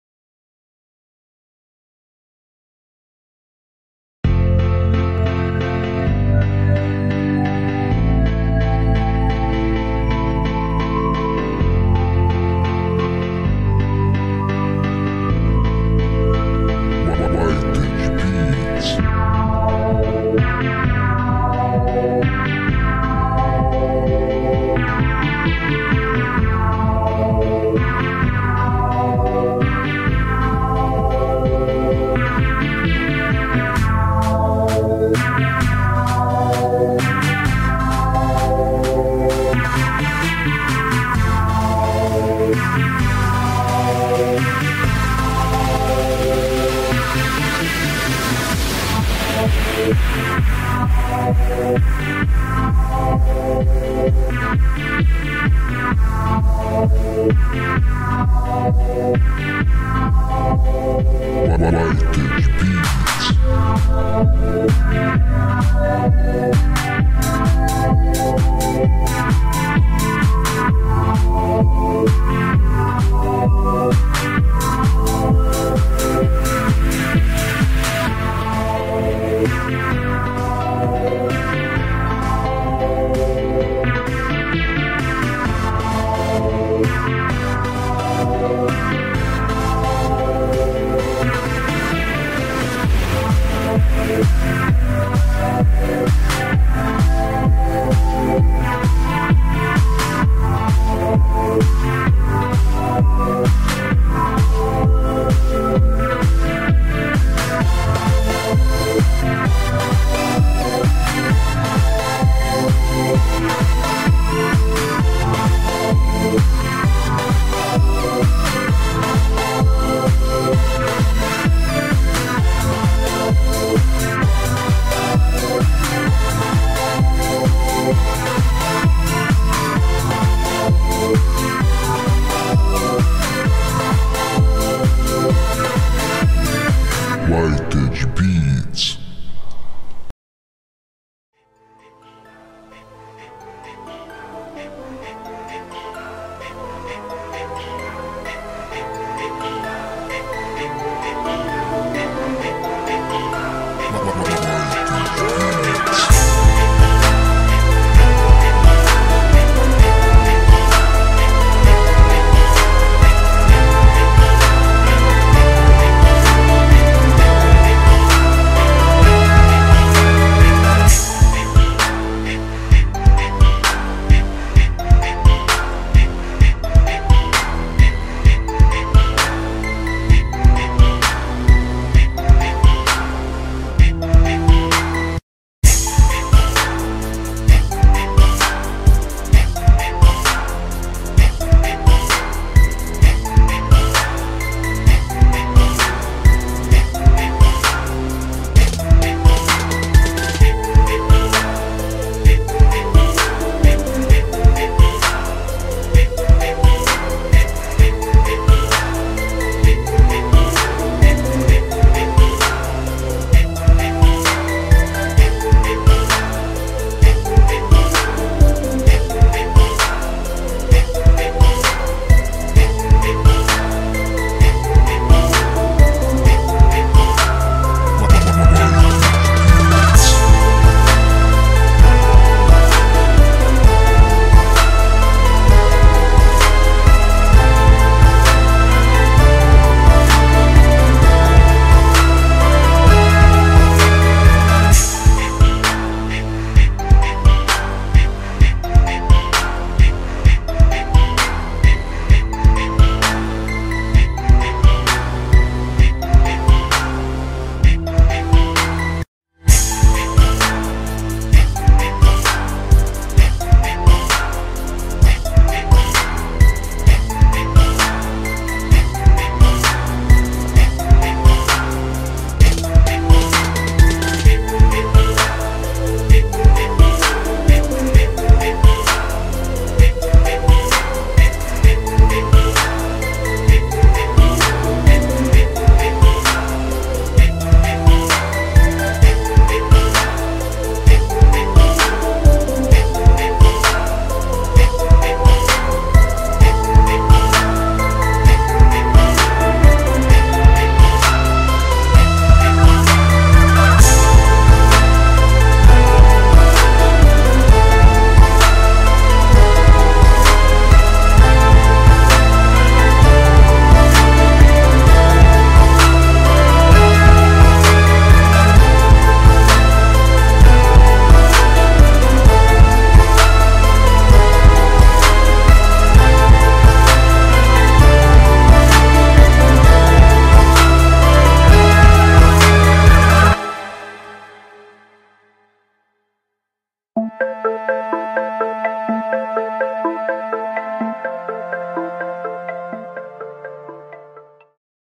Dessin rubiks cube 3D/Drawing 3D Rubiks cube
Heyo on se retrouve pour une nouvelle video, un nouveau speed drawing sur un rubiks cube avec un effet, une impression 3D. Qu'en pensez vous ?